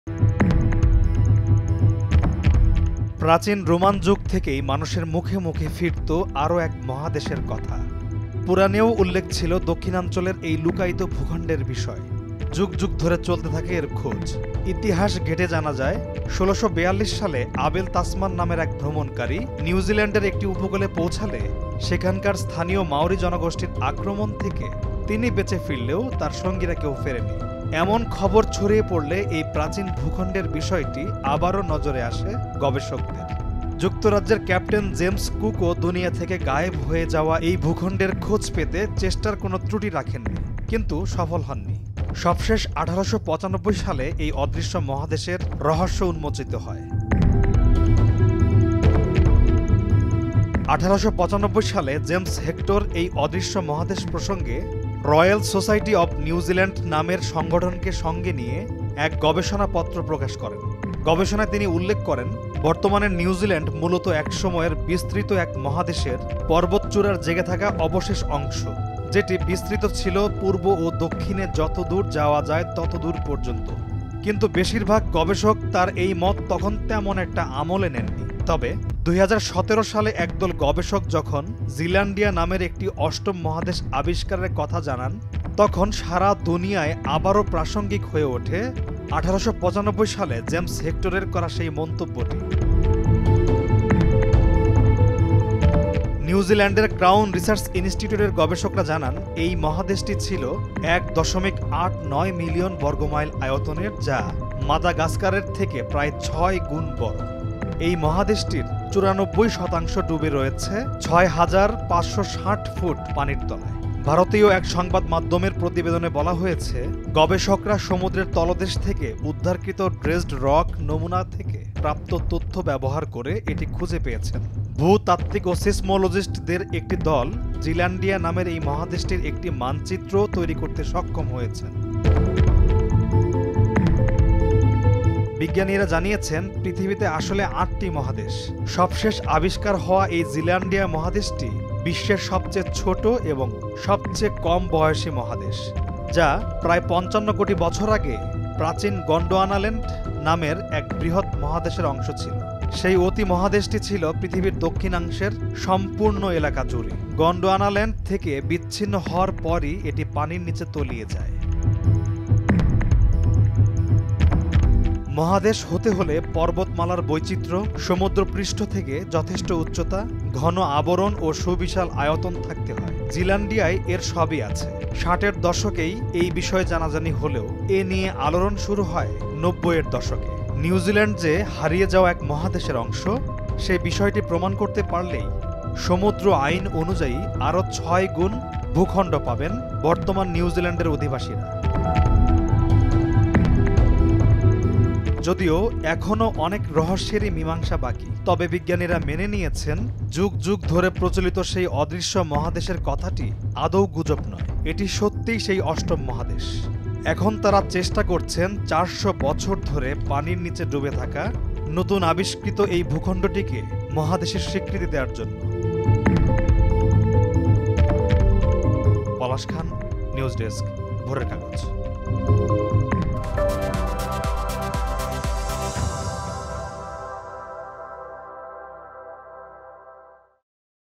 ৪০০ বছর ধরে সাগরের নিচে ডুবে রয়েছে পৃথিবীর অষ্টম মহাদেশ!
৪০০ বছর ধরে সাগরের নিচে ডুবে রয়েছে পৃথিবীর অষ্টম মহাদেশ!